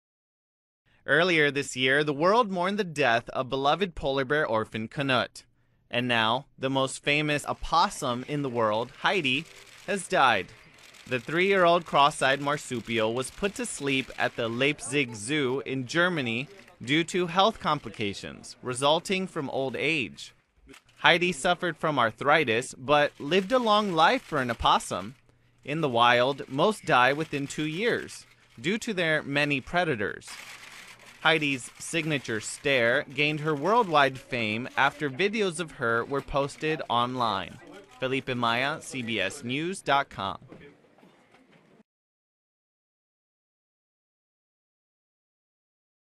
CBS News The Feed - Heidi, the cross-eyed opossum, is dead
Germany's famous cross-eyed opossum died of health complications due to old age. Felipe Maya reports.